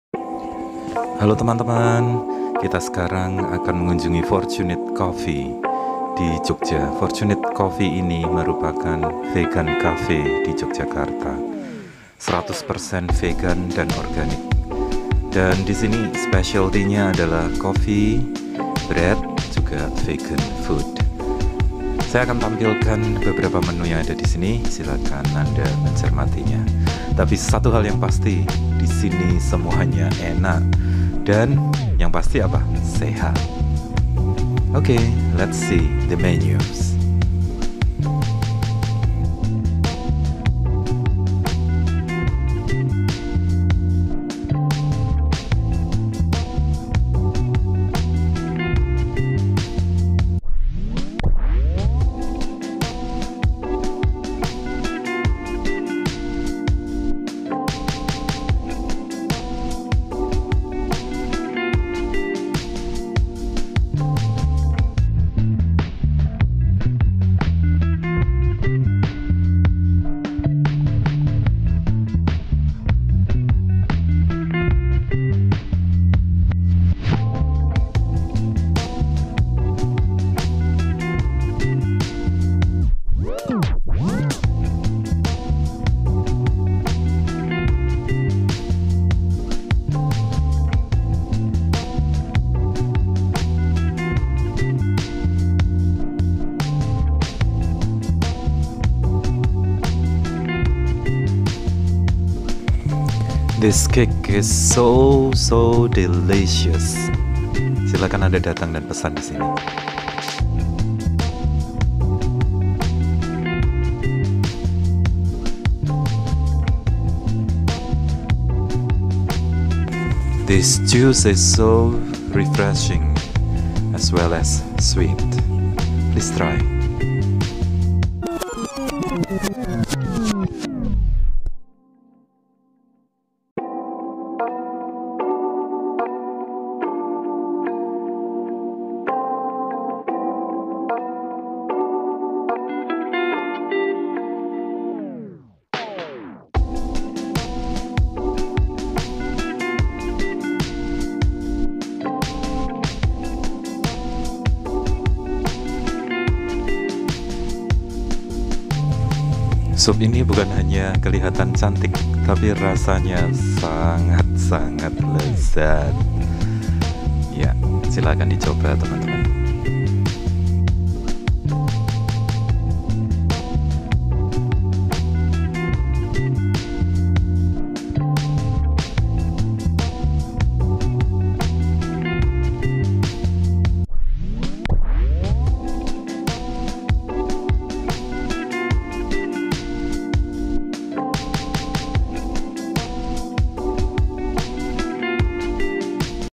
Fortunate Coffee Jogja
Fortunate Coffee Jogja merupakan reasyiran Vegan yang ada di Jogja, 100% organik, dengan kopi yg sangat istimewa roti dan maknan vegan

bahi anda yang ingin menikmati masakan sehat dan lezat di sinilah tempat yang tepat untuk dikunjungi

alamat
Jalan Jogonegaran no.49 E,tlp. ,(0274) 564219